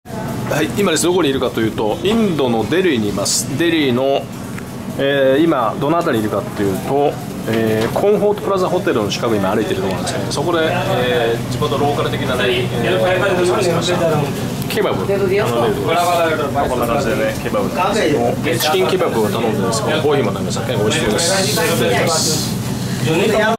アキーラさん堪能①インド・デリー・地下鉄Nehru Place 駅近くの軽食屋！New-Delhi in Delhi in India
デリー・地下鉄Nehru Place 駅近くの軽食屋！美味でした！

平成28年(2016年）3月8日に日本を発ってから、インドのムンバイに到着。ムンバイ→ゴア→コーチン→ムナール→チェンナイ→バラナシ→デリー→アーグラー→アーメダバード→ムンバイと25日間滞在。